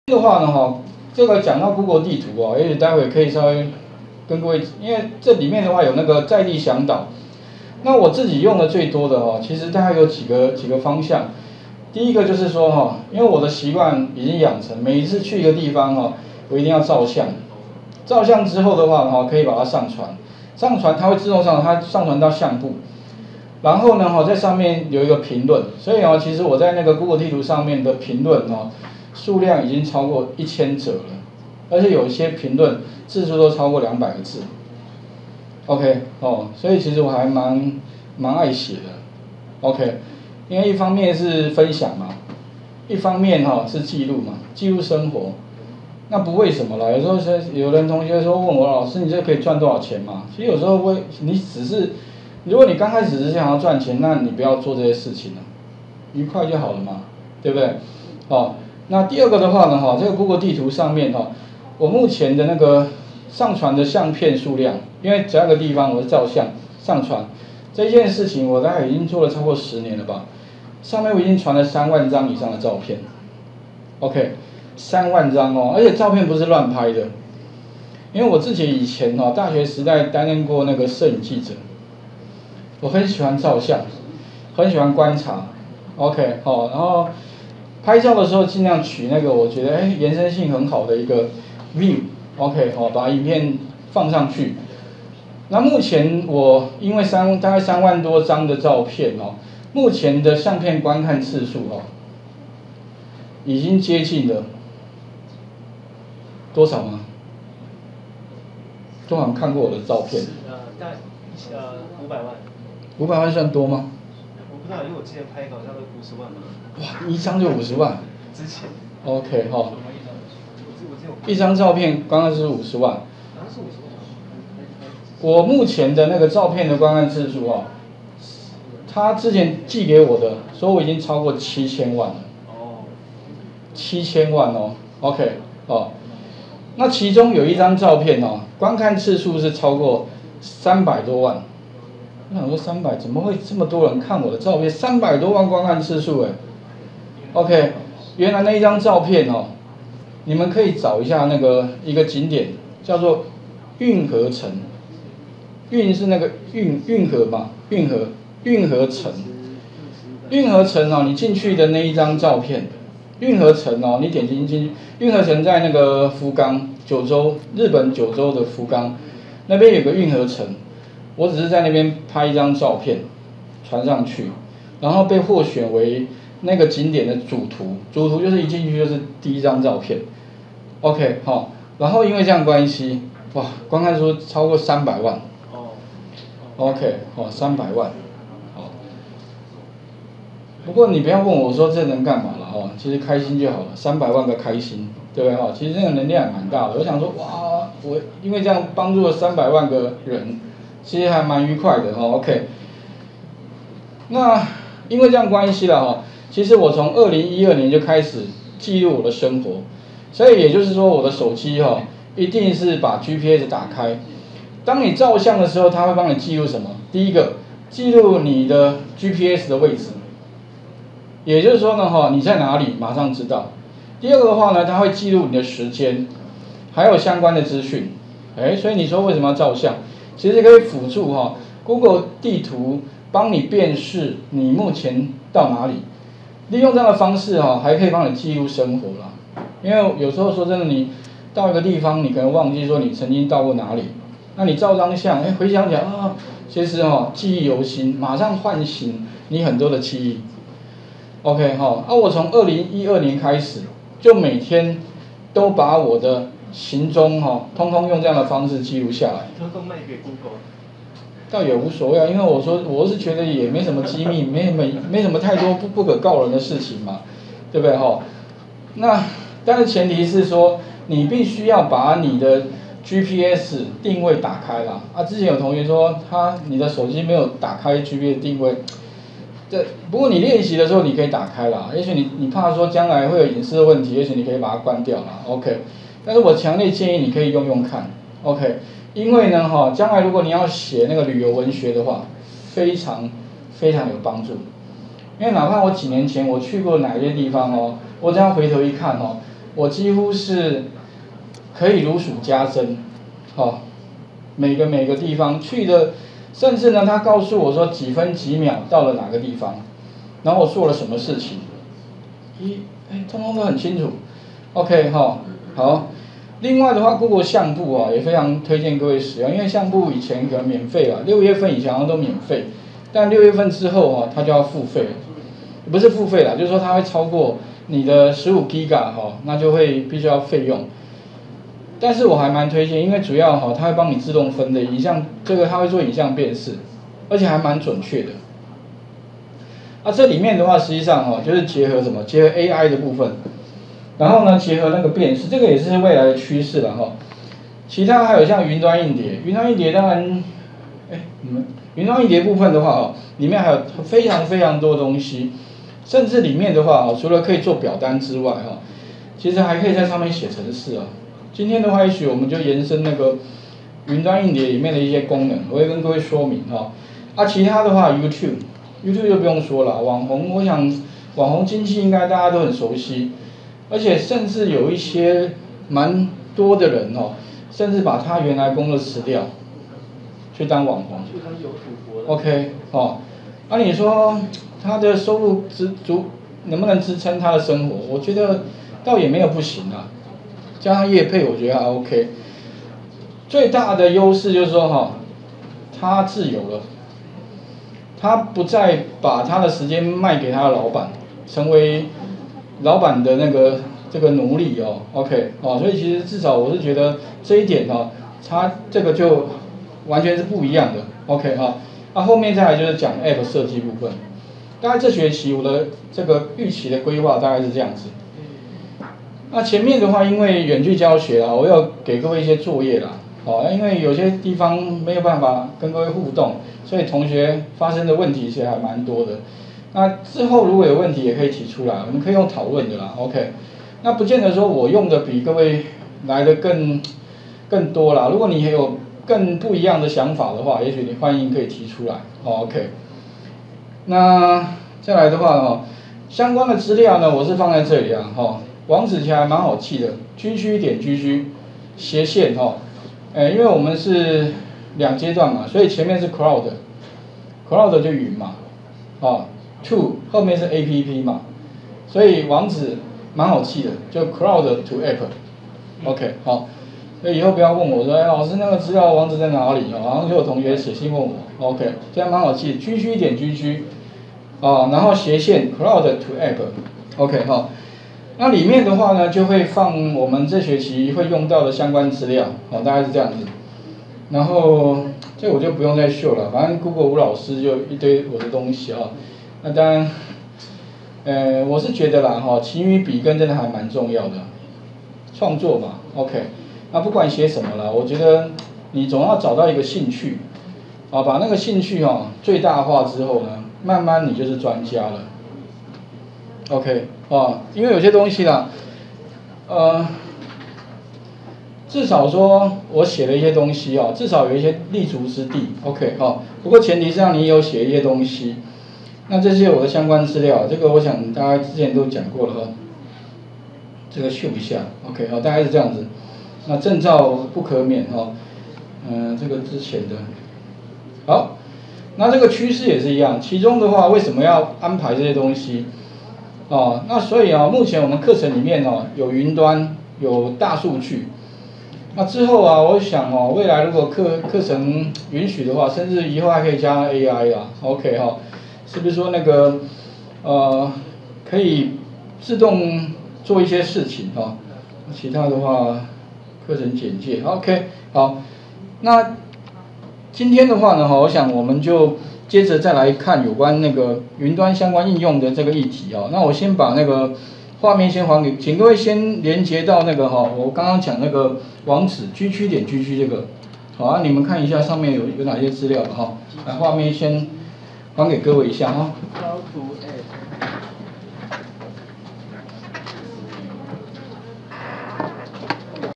01 課程介紹與雲端應用
元智_雲端應用與APP設計第4、5次上課

上課內容：
01_課程介紹與雲端應用
02_GOOGLE地圖時間軸與上傳試算表練習
03_雲端硬碟的試算表功能

01_GOOGLE試算表函數說明
02_用CODE判斷中英文與去除空白
03_錄製GAS巨集與執行
04_易錯說明與插入按鈕

吳老師  110/10/25